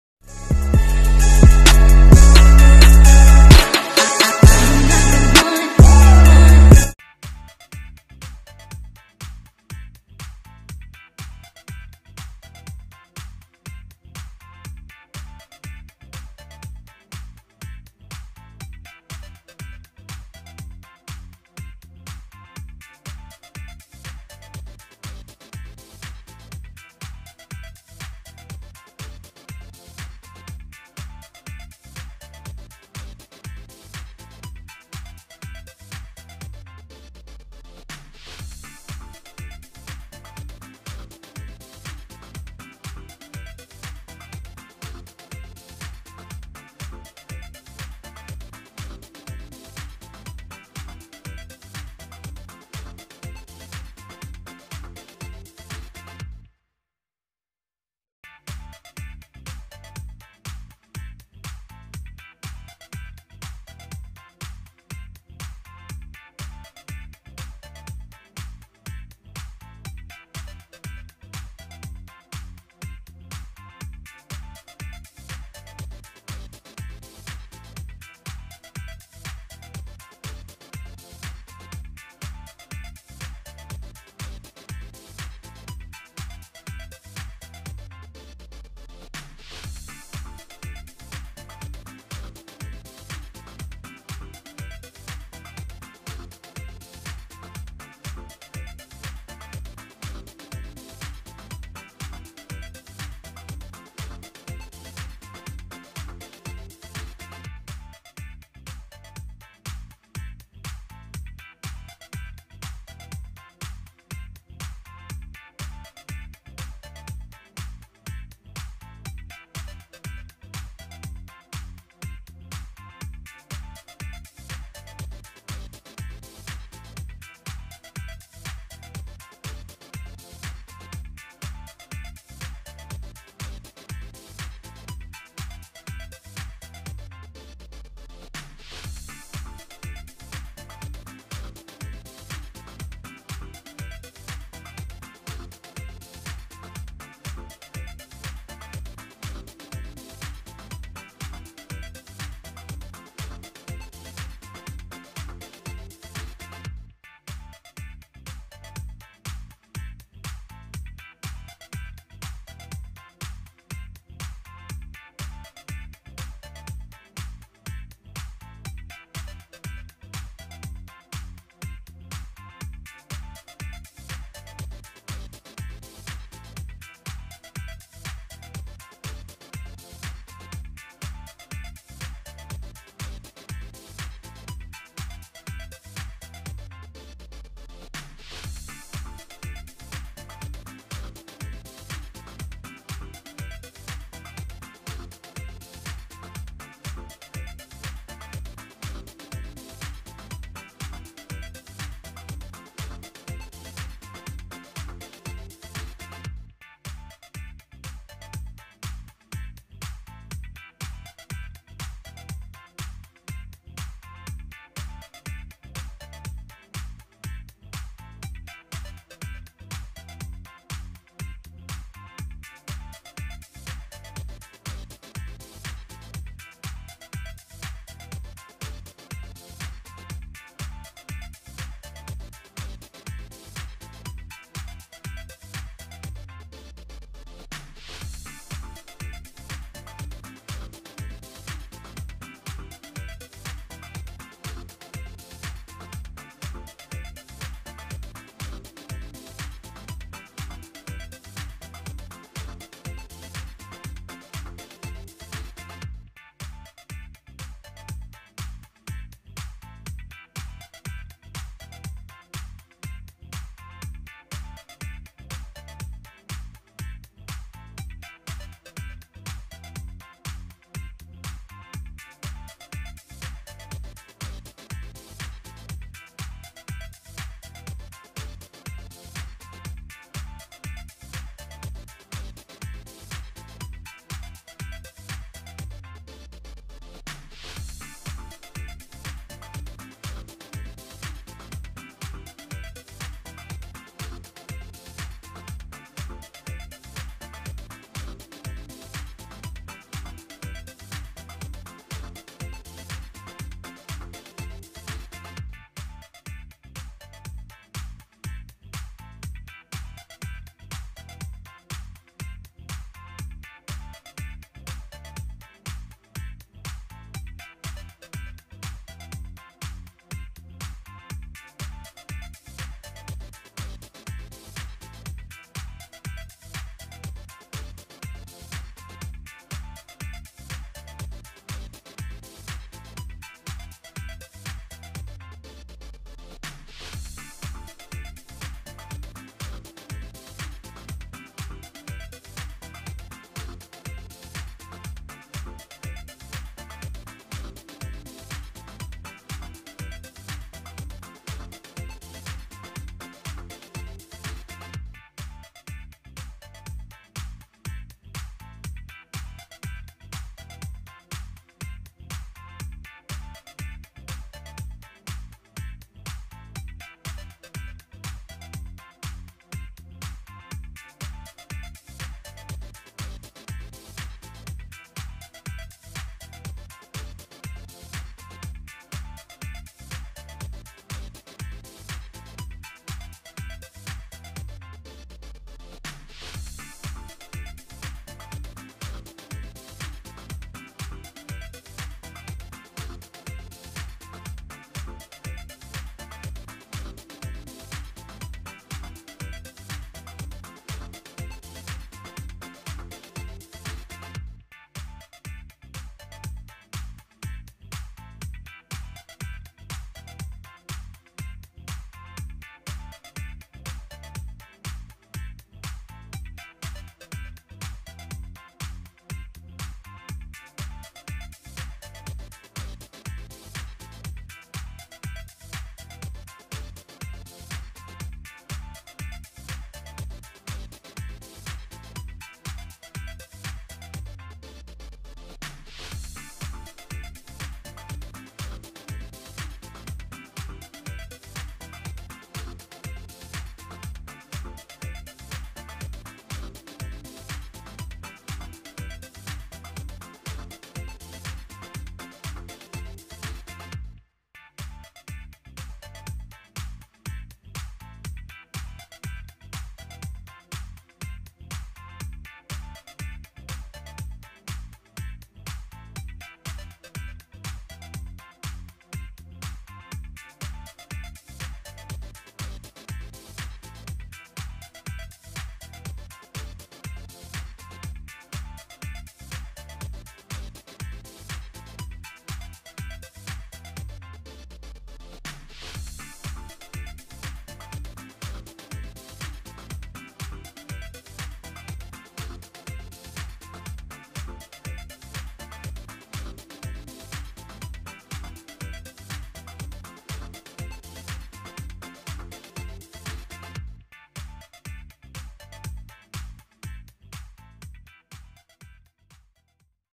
FULL FACE MAKE UP USING MIA COLLECTIONS (REVIEW)
I used the Mia Collections cosmetics as part of the review of her products. Maganda at magaan gamitin ang products. Pero kayo pa rin ang bahalang humusga! hahaha